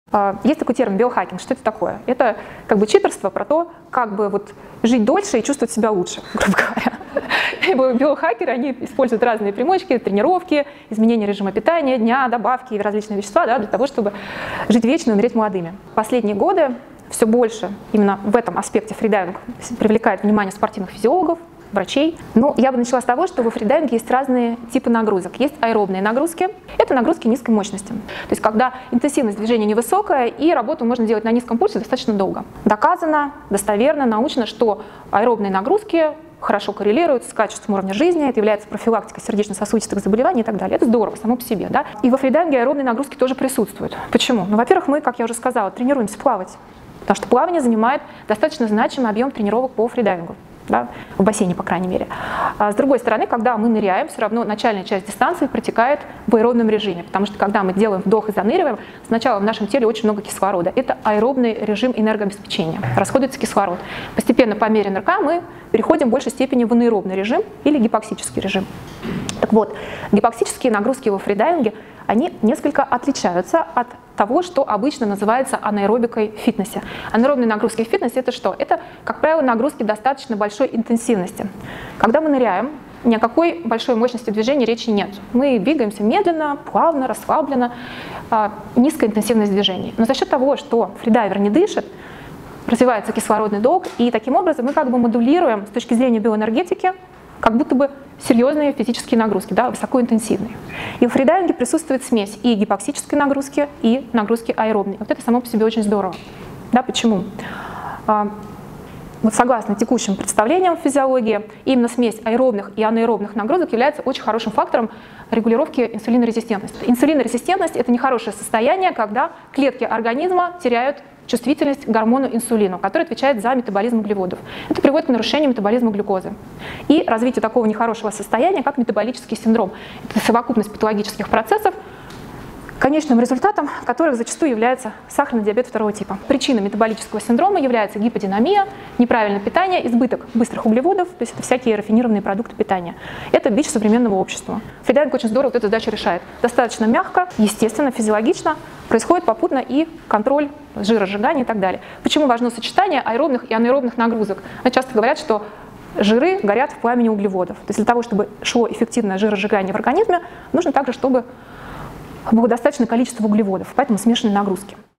WHY IS FREEDIVING A FORM OF BIOHACKING?
ПОЧЕМУ ФРИДАЙВИНГ ЯВЛЯЕТСЯ ОДНОЙ ИЗ ФОРМ БИОХАКИНГА?

Olga Davydova - SSI Freediving Instructor Trainer, Yoga Teacher, Freediving WR holder, Founder of Freediving &Yoga school Freebalance
- will tell you a lot of interesting info about freediving.

Один из лучших инструкторов по фридайвингу - Ольга Давыдова - расскажет много интересного о фридайвинге.